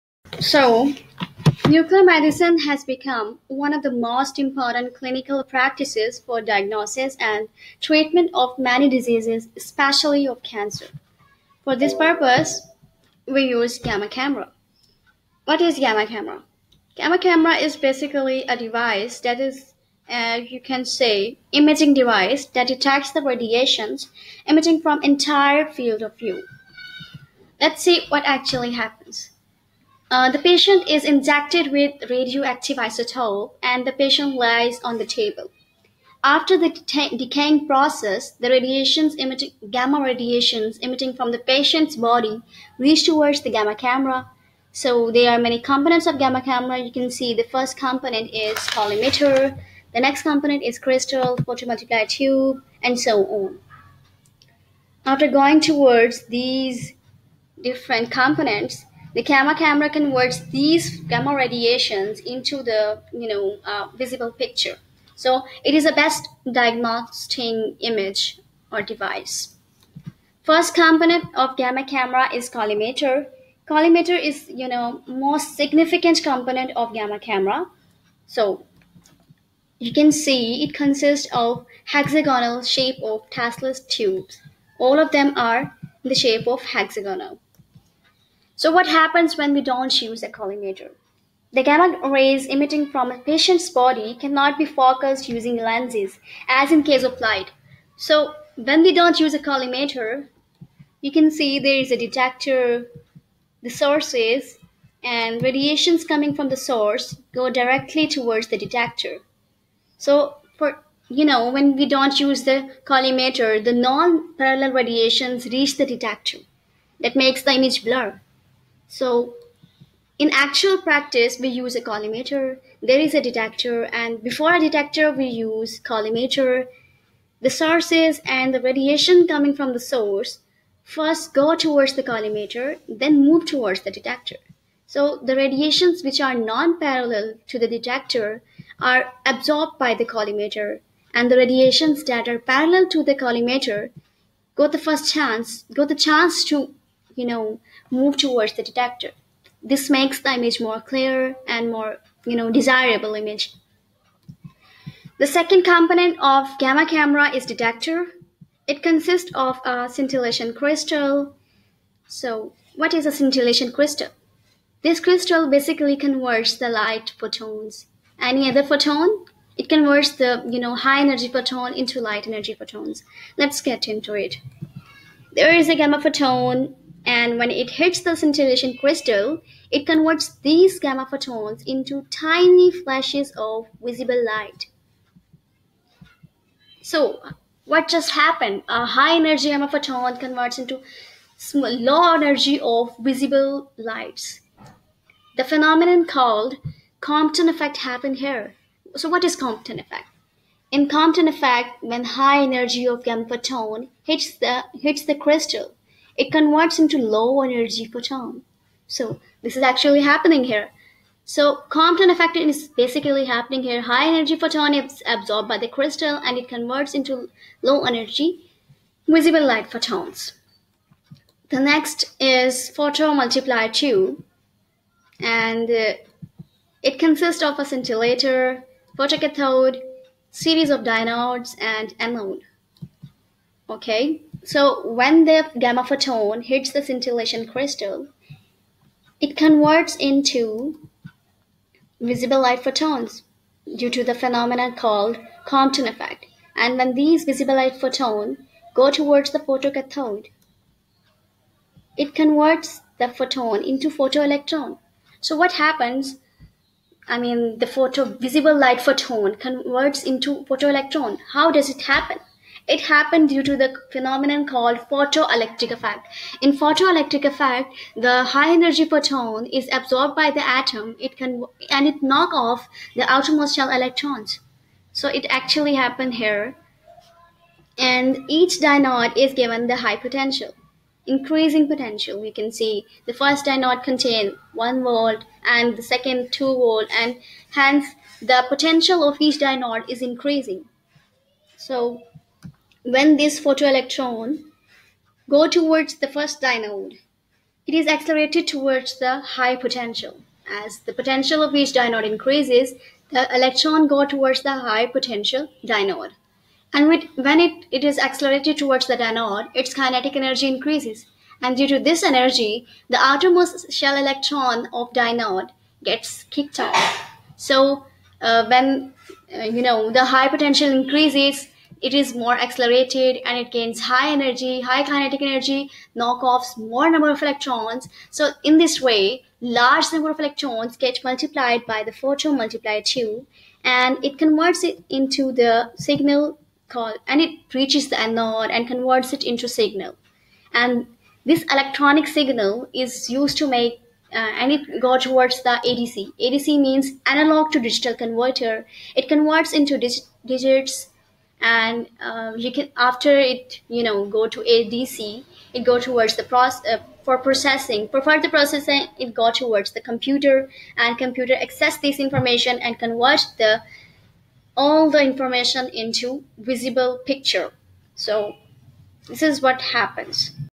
Gamma Camera
This video is about the components of Gamma Camera that is used in Nuclear Medicine